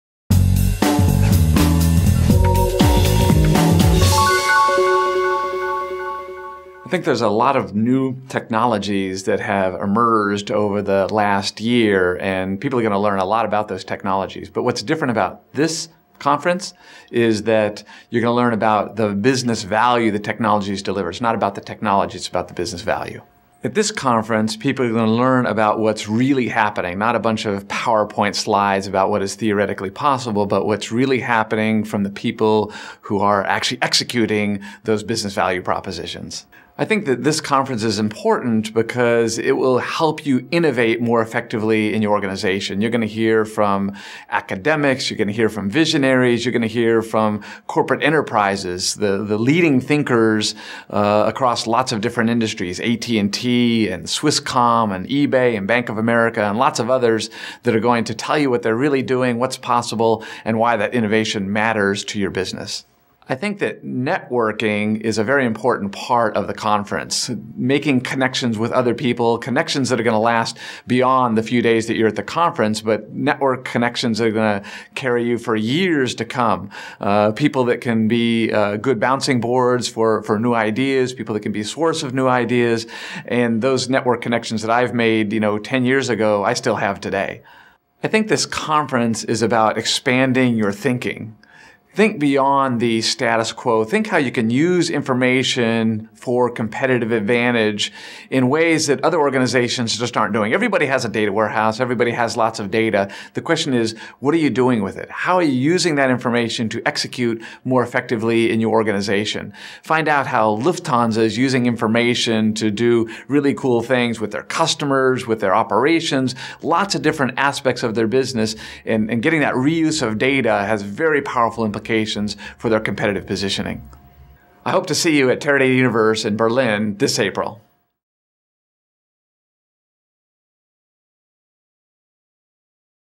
Teradata_Interview_with_Stephen_Brobst.mov
Interview with Stephen Brobst, Chief Technology Officer - Teradata Corporation.